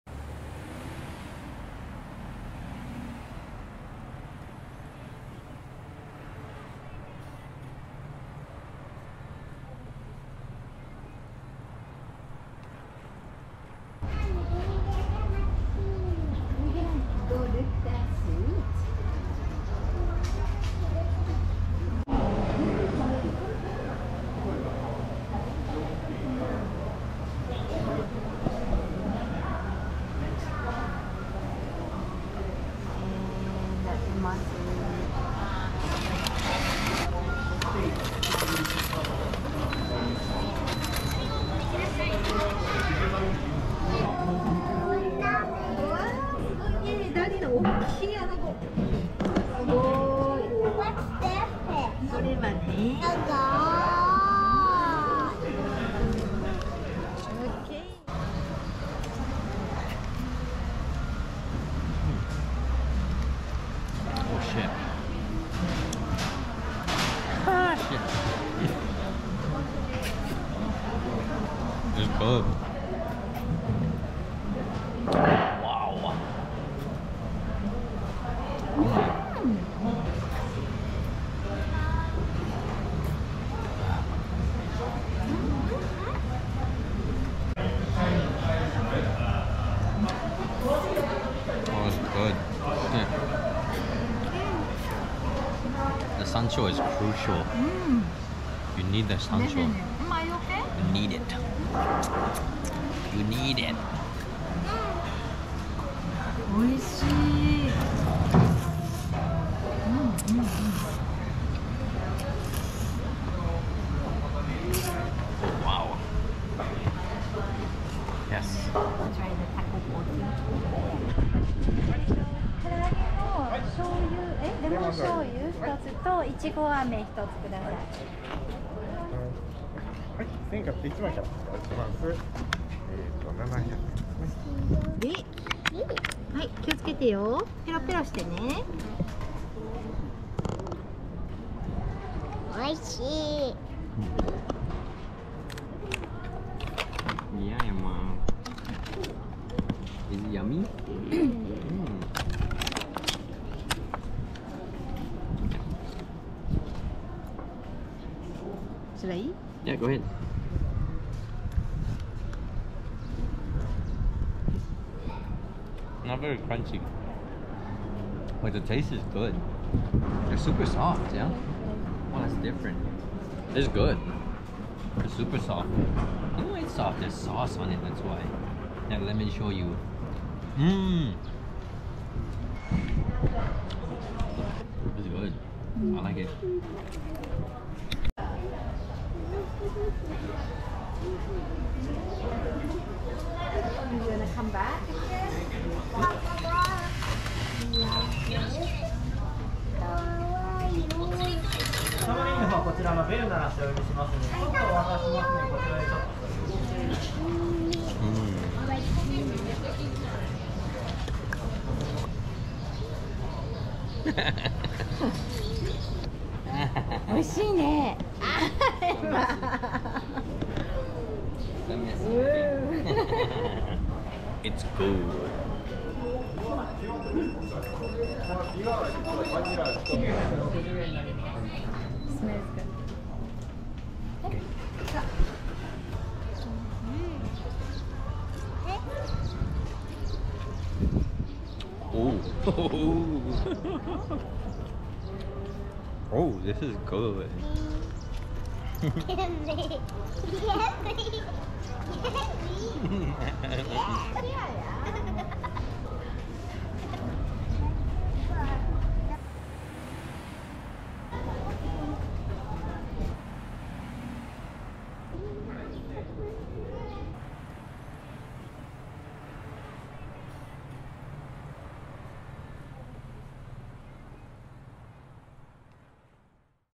Gourmet Japanese cuisine at a Roadside Station
We stopped by a popular roadside station on Awajishima, Japan. The food is gourmet and the view is spectacular.